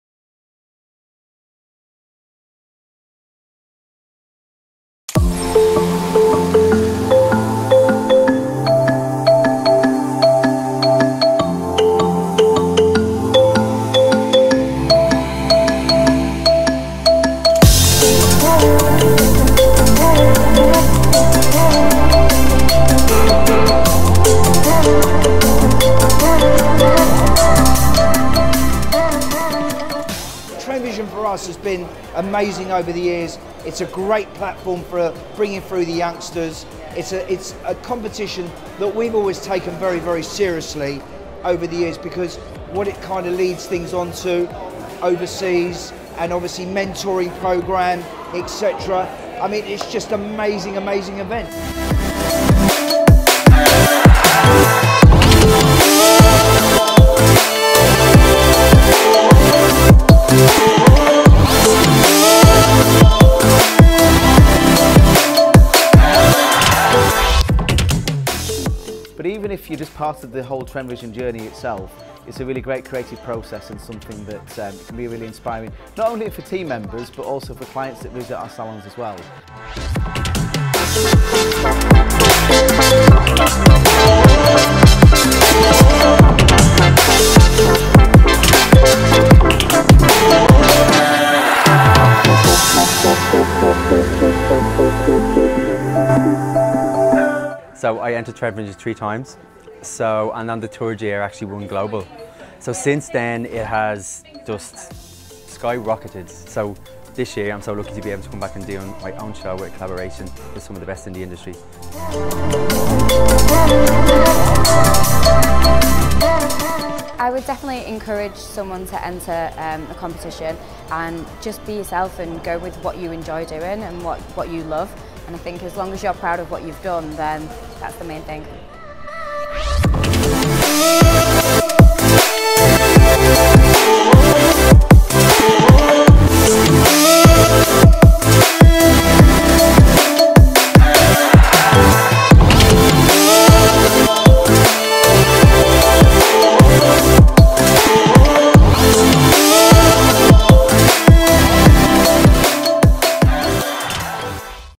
MyTrendVision: TVA 2019 Spectacle Throwback
For TrendVision Award 2020, we have partnered with Creative HEAD to film our new MyTrendVision digital campaign featuring six spotlight competitors and four judges. In this video we are reliving the Final of the TrendVision Award 2019 as we catch up with familiar faces hoping that we will get to experience the TrendVision Award live competitions and inspirational shows in 2021. To find out more about Wella Professionals TrendVision Award go Follow @creativeheadmag and @wellahairuki on Instagram and @wellaUKI on Facebook to see more videos in this series.